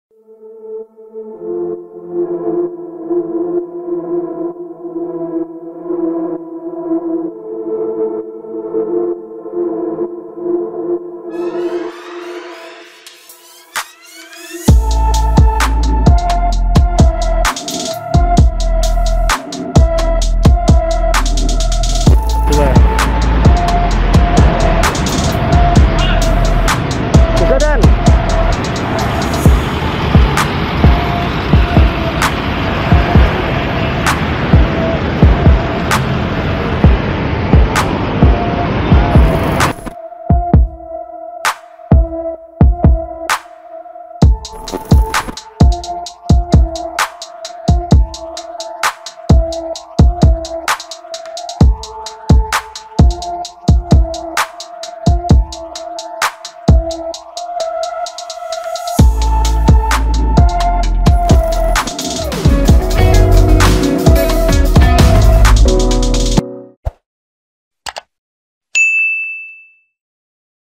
Mobil Mewah Rusak Parah Usai Tabrak Separator Busway
Sebuah video memperlihatkan mobil mewah mengalami kecelakaan tunggal di depan Gedung DPR/MPR, Jakarta Pusat, Rabu (23/2/2022).

Dalam video terlihat roda depan sebelah kanan mobil terlepas usai menabrak separator busway.
Video terkini dan terbaru..